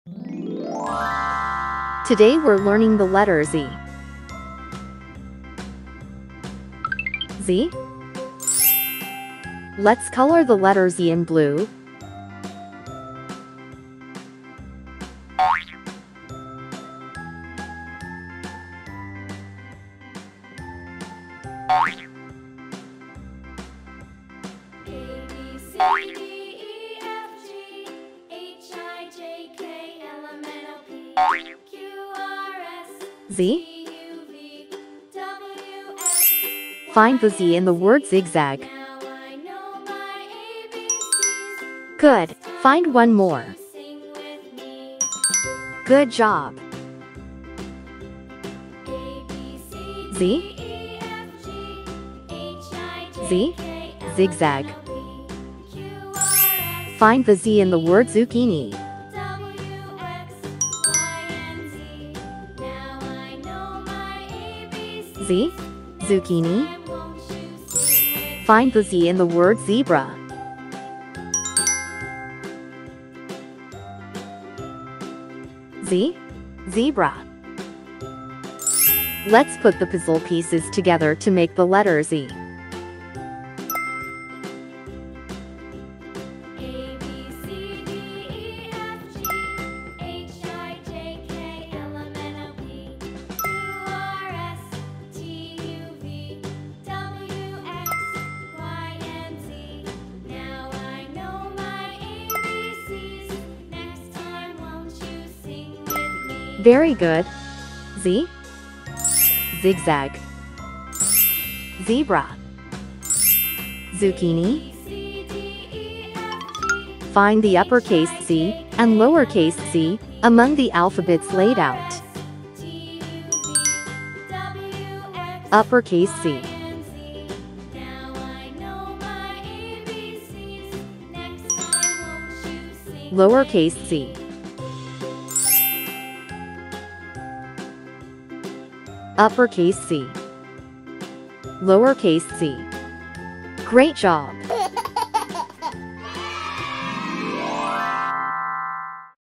Learn ABC for Kids | Learning the alphabet by matching puzzle pieces | alphabet coloring page
Hello, Mom and Dad!
Today we learn the alphabet 'Z'.
Learn words that start with Z: zebra, zucchini, zigzag.
What alphabet will it be if you put the puzzle pieces together?
Find the uppercase 'Z' and lowercase 'z' among the various alphabets, and write the alphabet along the dotted line.

Please click 'Like'. It is a great help in video production.